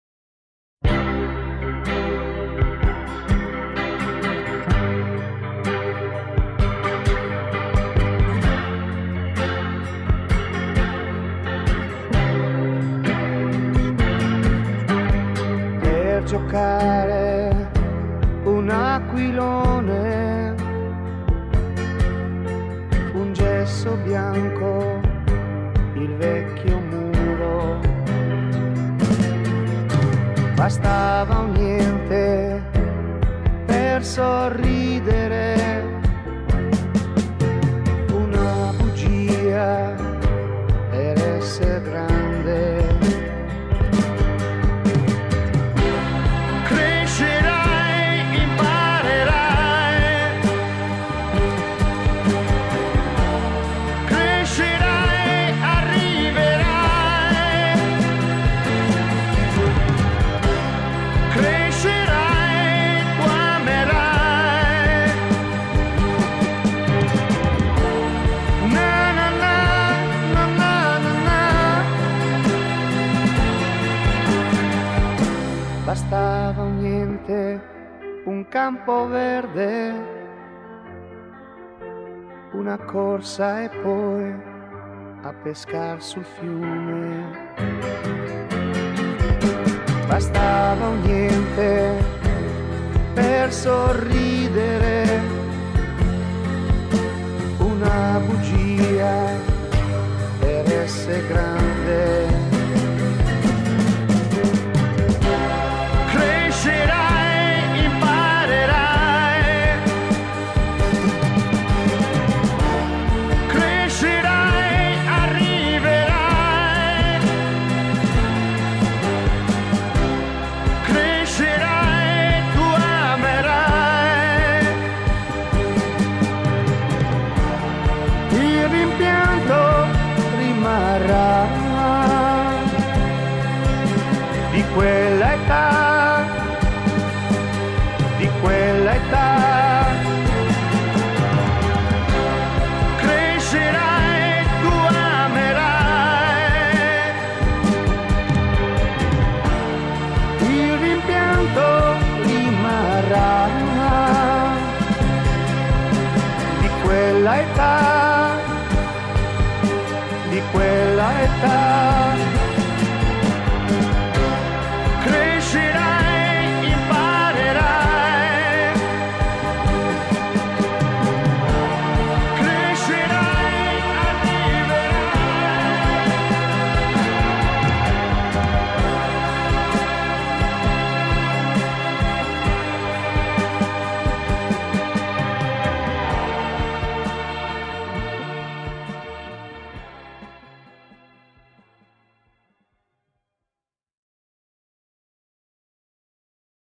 Mama Africa 2016 - Mémoires
In solo tre minuti l'attività di Mama Africa onlus svolta nel corso del 2016. Sono ben 15 i progetti che l'associazione umanitaria porta avanti grazie al sostegno di migliaia di amici.